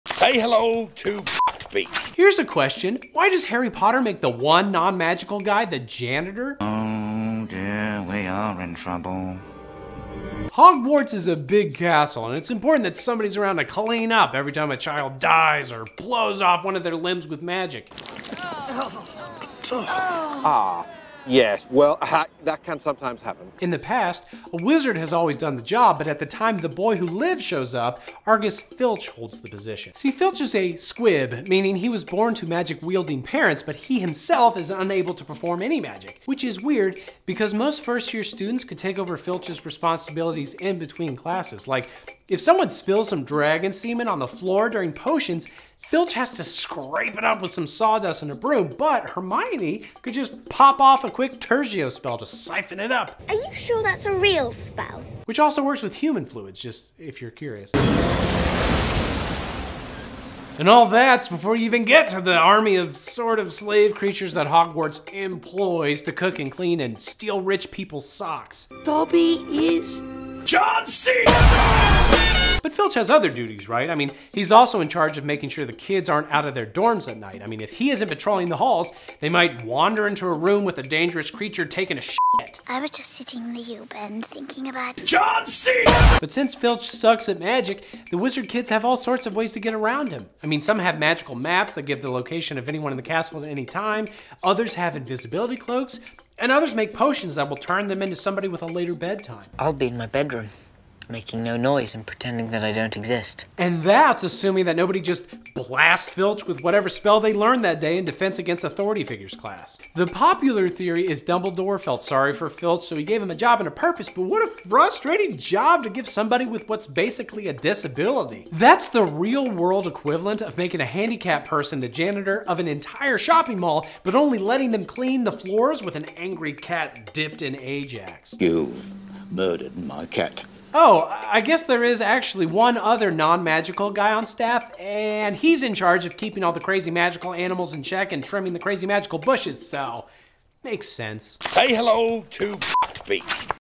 Why Is The Non-Magical Guy Forced To Be The Janitor?
Muggle employees get the short end of the stick at Hogwarts.